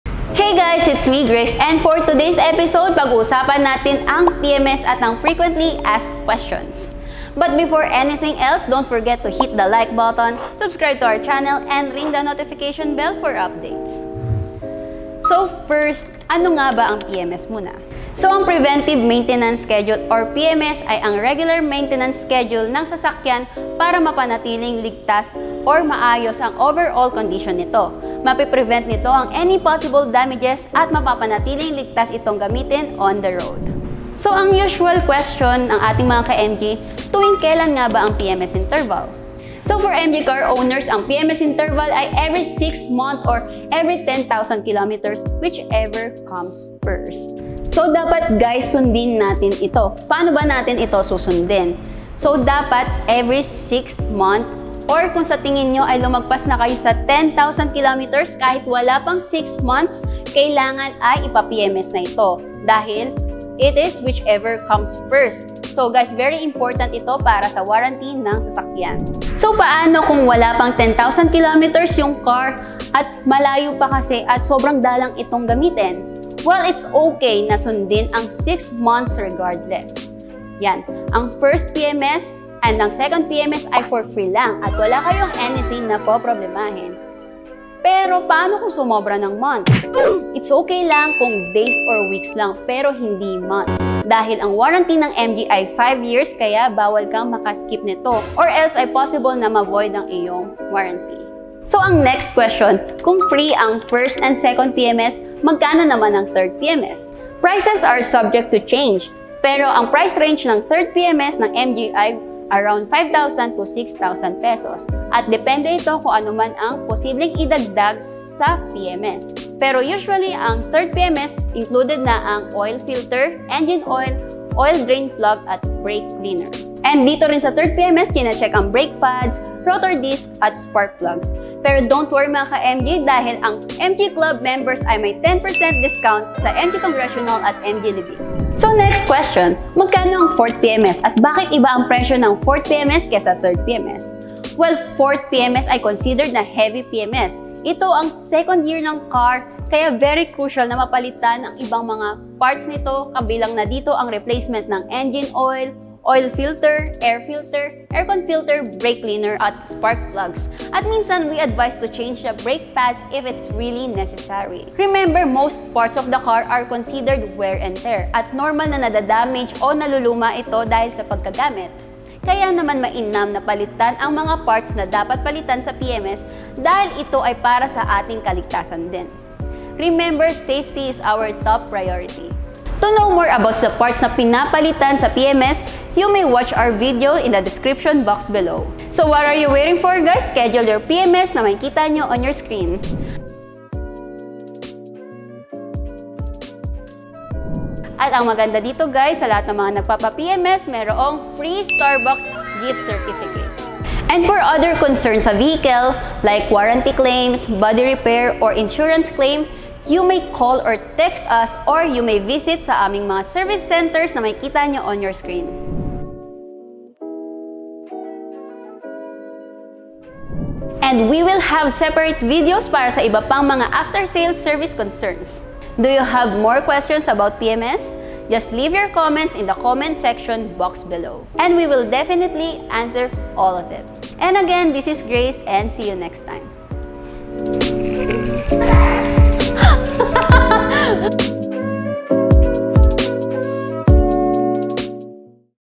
Preventive Maintenance Schedule (PMS) Frequently Asked Questions
Ano ang PMS? Tuwing kailan nga ba dapat magpa-PMS? Ano ang PMS interval ng MG cars? Alamin ang ilang mga PMS Frequently Asked Questions  sa video.

Book your PMS with us and get a free Starbucks Gift Certificate!

MG Congressional:
Address: MG Congressional, 16 Congressional Ave. cor. San Beda St., Brgy. Bahay Toro, Quezon City.
Phone/ Viber / WhatsApp/ Telegram: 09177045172

MG Libis:
Address: Blk. 11 E. Rodriguez Avenue, Bagumbayan Libis, Quezon City